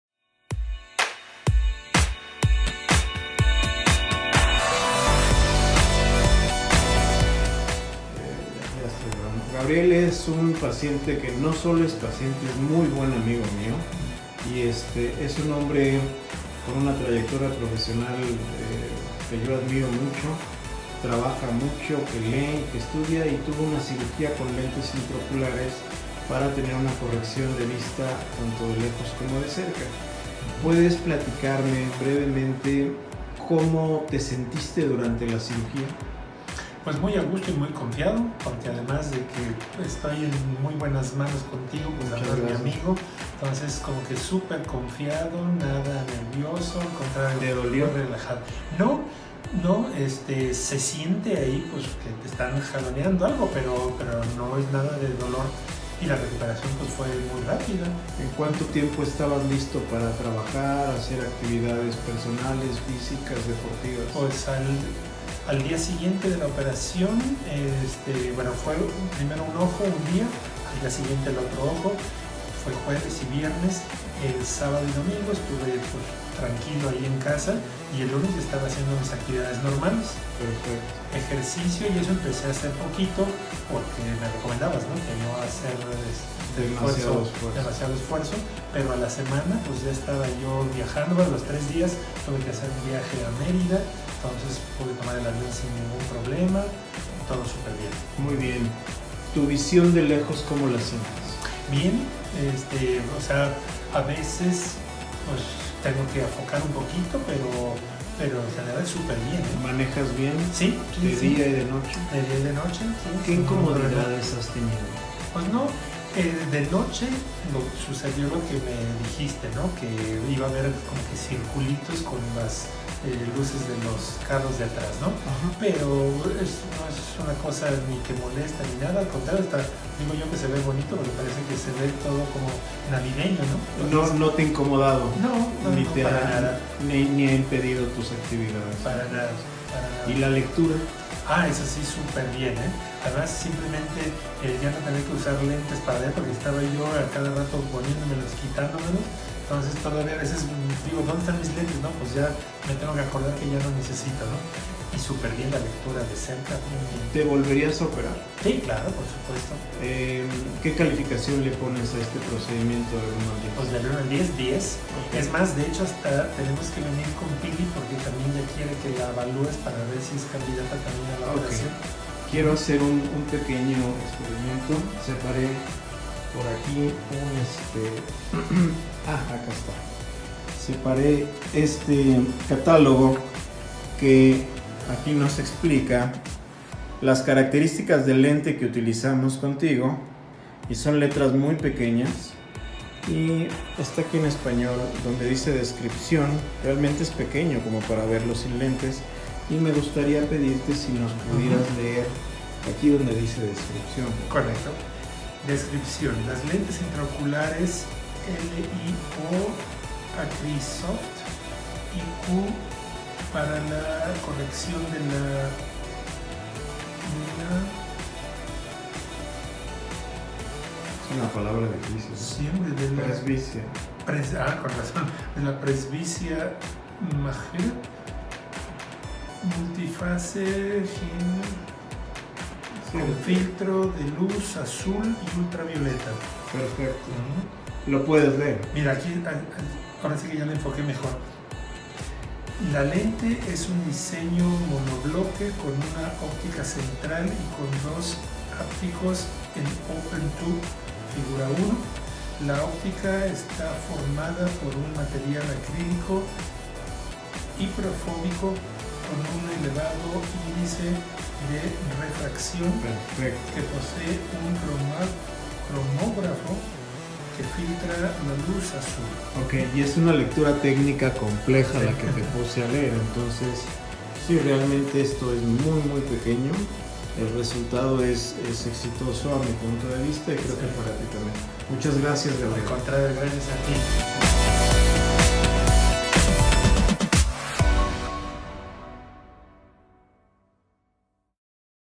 Testimonio de cirugía de catarata con lentes intraoculares.
Gabriel nos explica con lujo de detalles la experiencia de una cirugía de cataratas en ambos ojos con lentes intraoculares multifocales para presbicia.